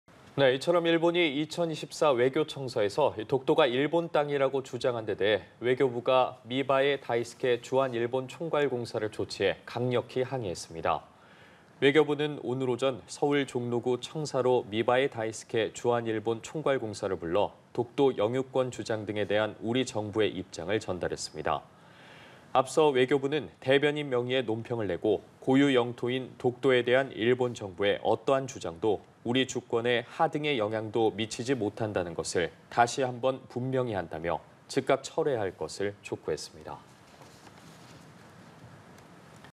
정부, '독도는 일본땅' 日 주장에 총괄공사 초치 / 연합뉴스TV (YonhapnewsTV)
정부, '독도는 일본땅' 日 주장에 총괄공사 초치

일본이 2024 외교청서에서 독도가 일본땅이라고 주장한 데 대해 외교부가 미바에 다이스케 주한일본 총괄공사를 초치해 강력히 항의했습니다.

외교부는 오늘(16일) 오전 서울 종로구 청사로 미바에 다이스케 주한일본 총괄공사를 불러 독도 영유권 주장 등에 대한 우리 정부의 입장을 전달했습니다.

앞서 외교부는 대변인 명의의 논평을 내고 "고유 영토인 독도에 대한 일본 정부의 어떠한 주장도 우리 주권에 하등의 영향도 미치지 못한다는 것을 다시 한번 분명히 한다"며 즉각 철회할 것을 촉구했습니다.